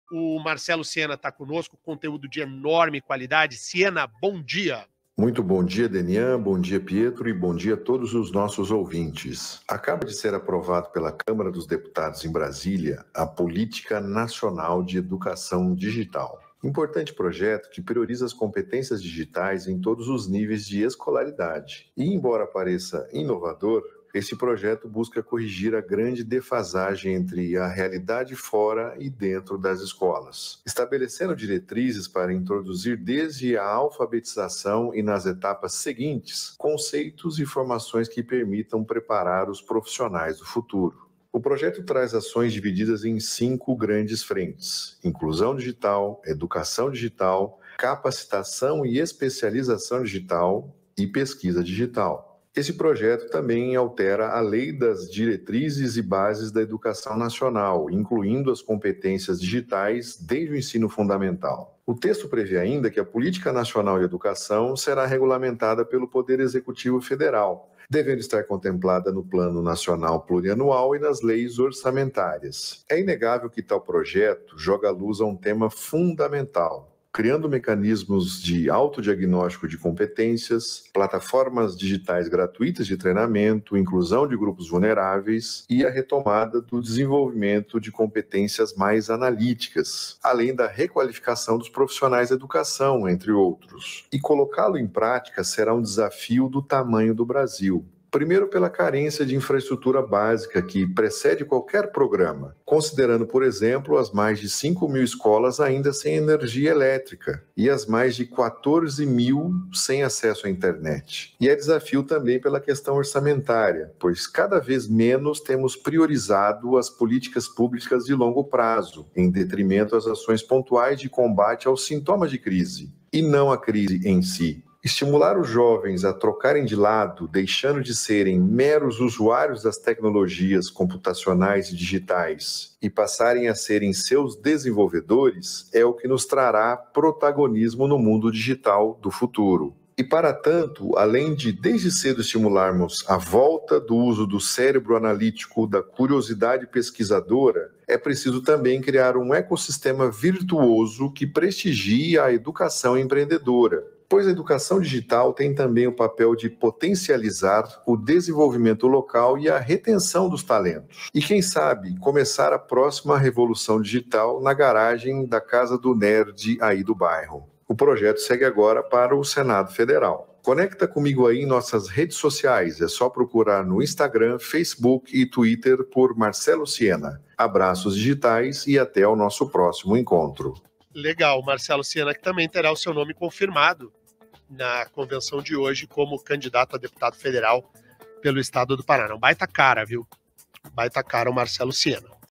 Marcelo Siena fala sobre projeto que cria política nacional de educação digital
Dra. Cristiane Paulin - Harmonização Orofacial